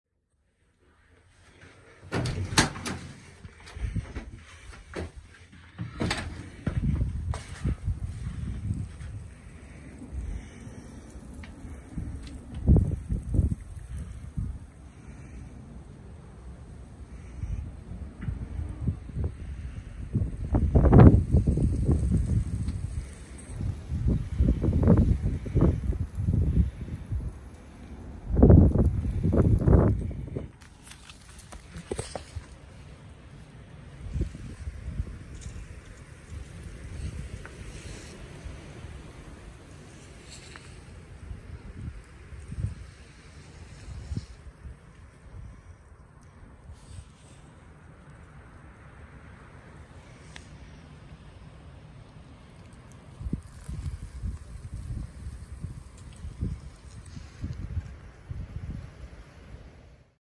Winter Storm Gail - 9 PM
Few hours has passed. Storm continues to rage on.